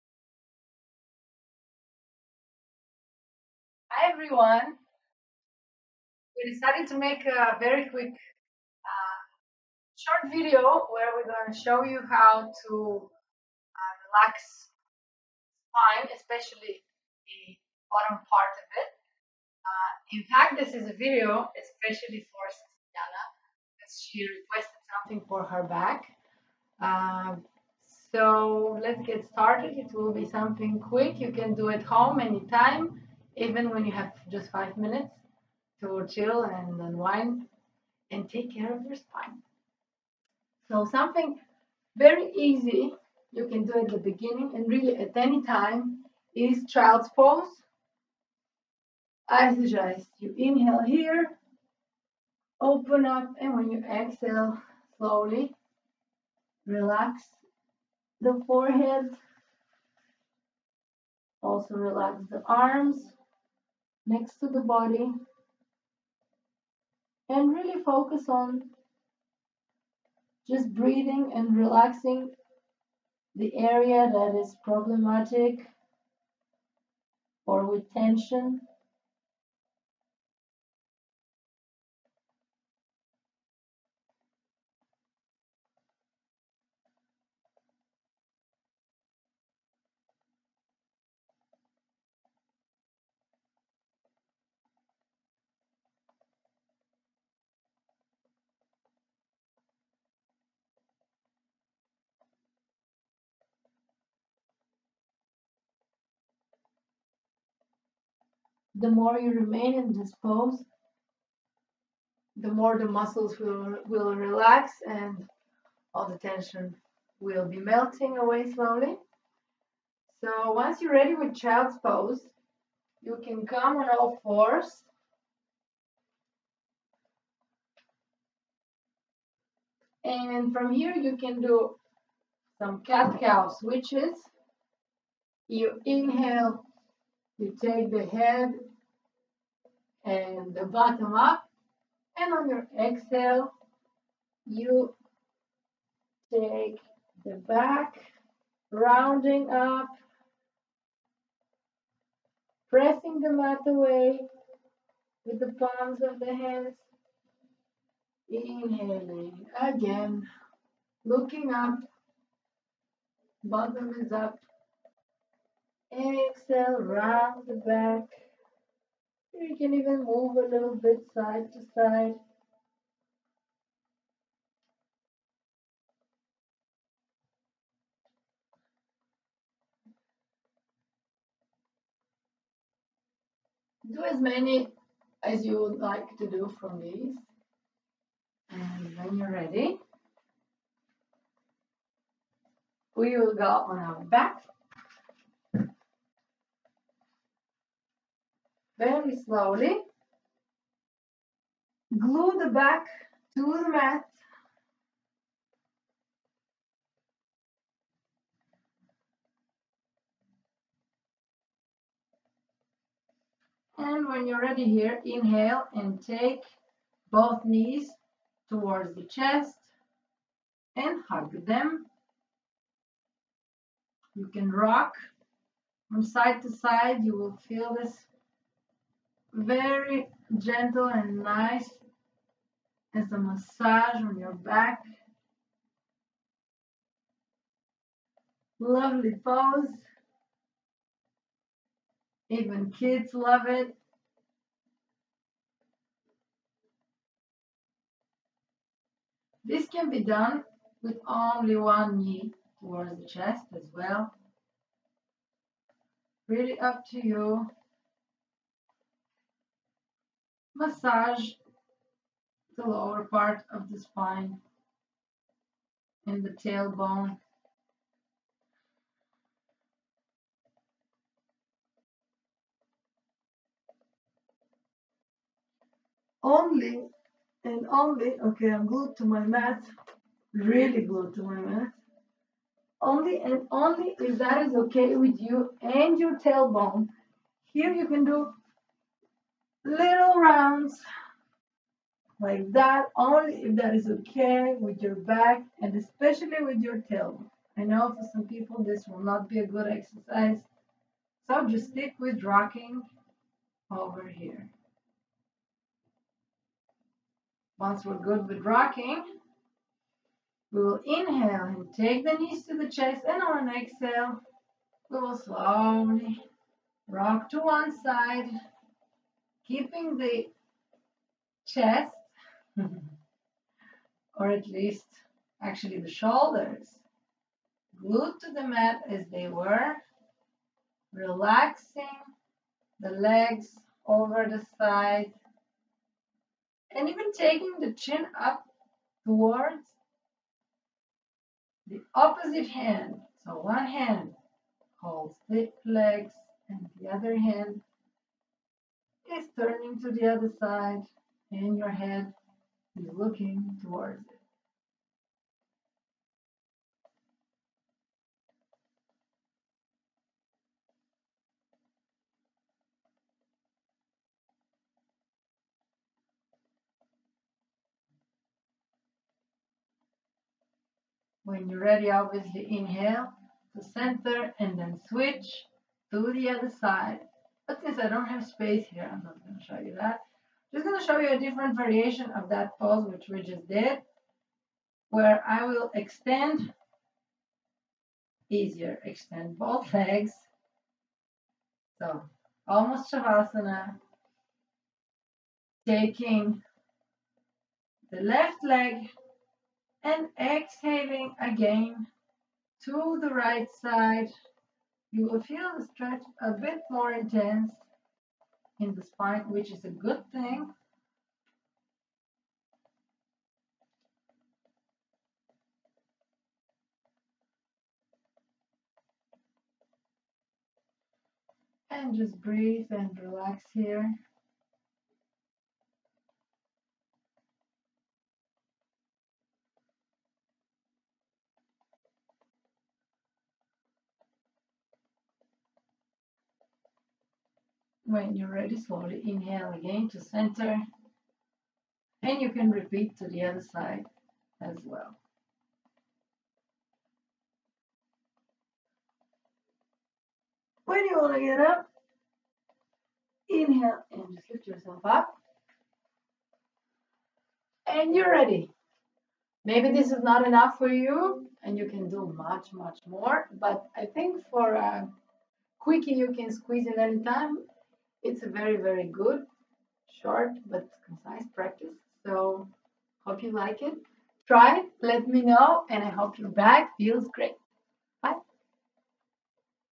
Yoga for lower back pain
Hey there yogis, we quickly filmed this to respond to a friend's request for some easy yoga for lower back pain. These easy to practice yoga poses are suitable for yoga beginners and will release the tension in your lower back, increasing your general well-being. Feeling good is just 10 minutes away! Make sure you take care of yourself and your spine :)
Namaste xo